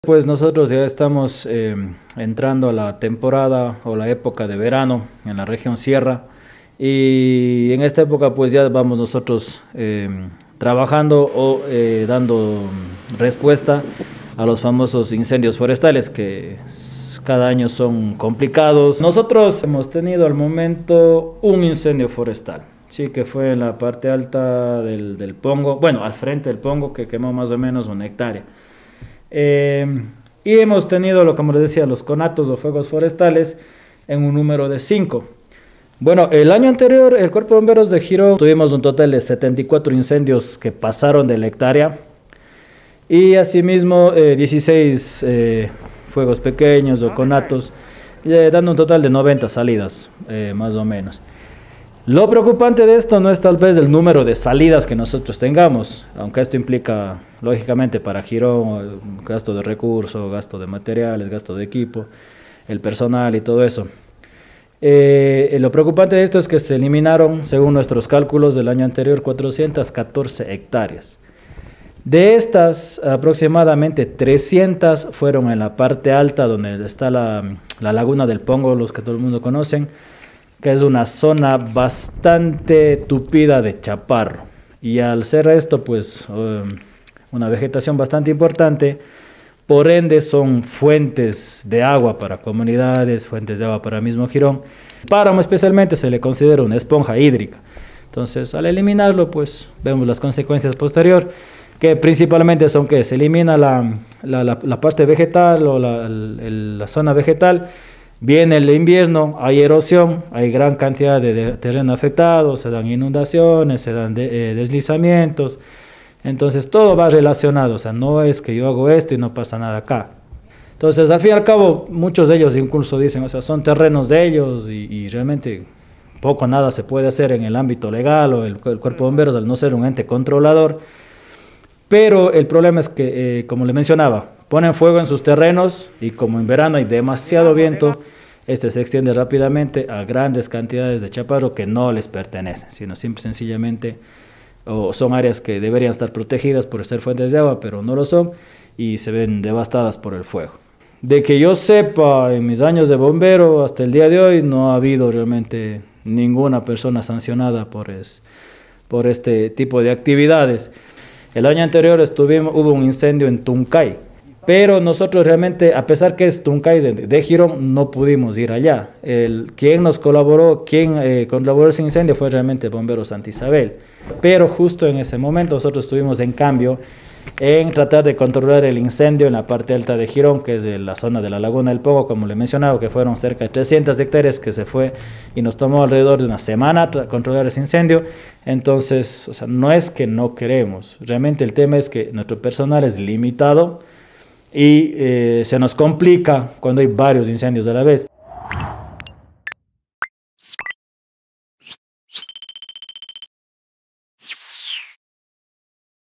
Cada año cientos de hectáreas de vegetación se vuelven a cenizas en Girón
La temporada de verano, para Girón, a más de una época para vacacionar y disfrutar al aire libre, es sinónimo de atentado y afección a la naturaleza; cada año, los bomberos y otros organismos deben luchar contra el fuego en múltiples lugares, cada vez más distantes y en zonas montañosas que poco a poco van perdiendo el bosque y vegetación nativa. Las lluvias aún no han cesado por completo pero, ya empezaron las quemas.